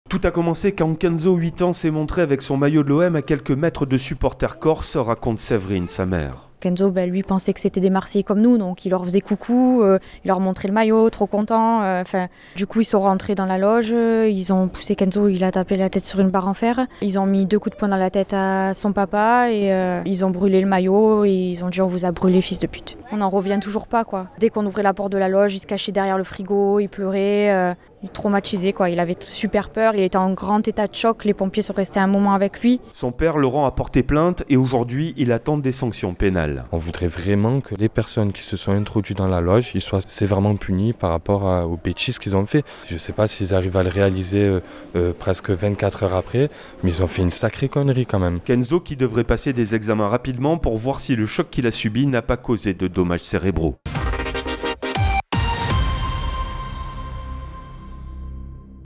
Ajaccio-OM : les parents de Kenzo témoignent de l'agression
RMC a recueilli le témoignage des parents de Kenzo, agressés dans leur loge lors du match de Ligue 1 Ajaccio-OM. Le garçon est passé du rêve au traumatisme. Une plainte a été déposée.